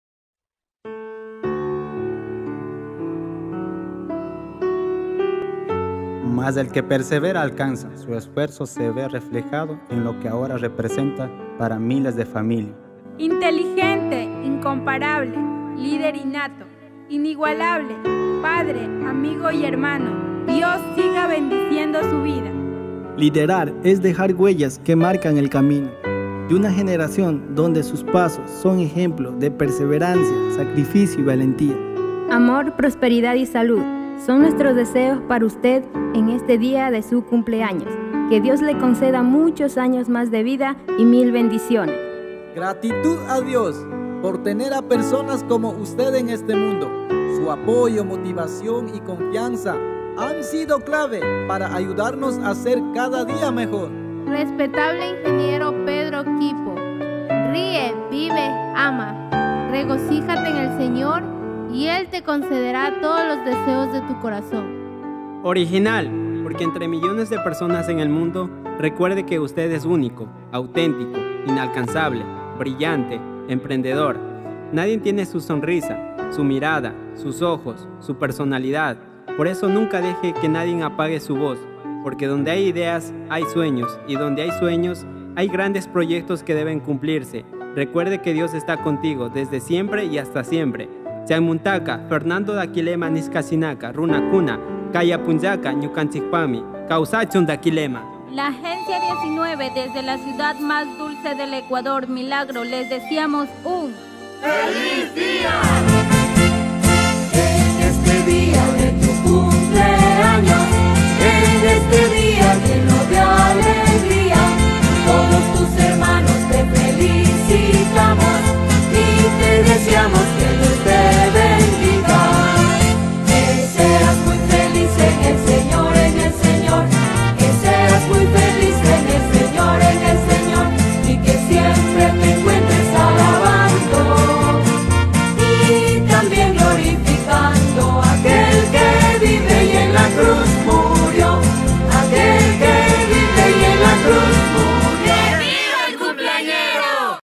FELIZ CUMPLEAÑOS ING PEDRO KHIPO - AGENCIA MILAGRO 24-07-2022
ADMIRABLE PRODUCCIONES
PRESENTA:"Homemaje al flamente cumpleañero Ing  Pedro Khipo"
Interpretación: Funcionarios Ag. Milagro/Coop. Daquilema
Sonido y Masterización: Ruiz Records
Cámara: Jpc Pro Hd Films
Edición: Sumak Life Studio/Juan Pablo Cando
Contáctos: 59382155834

PELICULAS COMPLETAS
PELICULAS NUEVAS COMPLETAS
PELICULAS DE ESTRENO
PELICULAS NUEVAS 2020